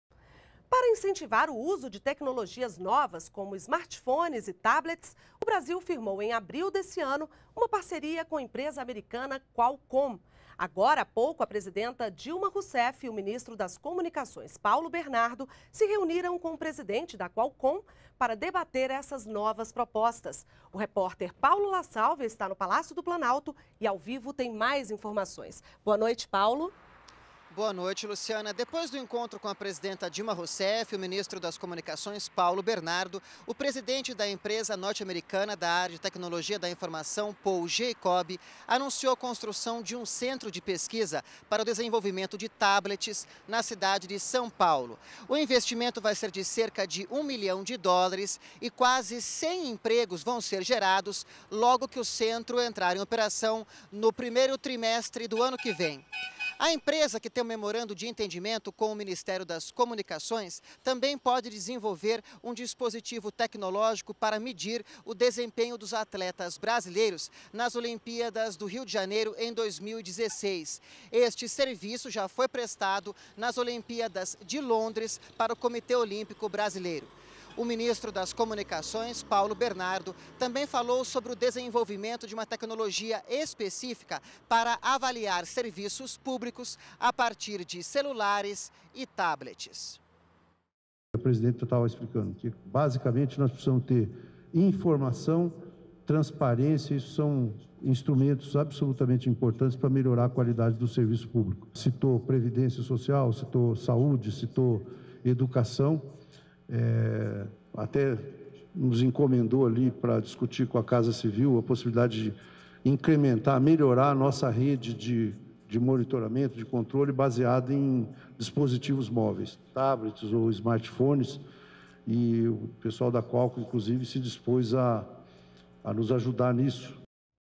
Governo brasileiro e empresa americana se reúnem para discutir uso de novas tecnologias
NBR NOTÍCIAS - 21.08.12: Para incentivar o uso de novas tecnologias como smartphones e tablets, o Brasil firmou, em abril desse ano, uma parceria com empresa americana Qualcomm. A presidenta Dilma Rousseff e o ministro das Comunicações, Paulo Bernardo se reuniram nesta terça-feira (21) com o presidente da Qualcom para debater novas propostas.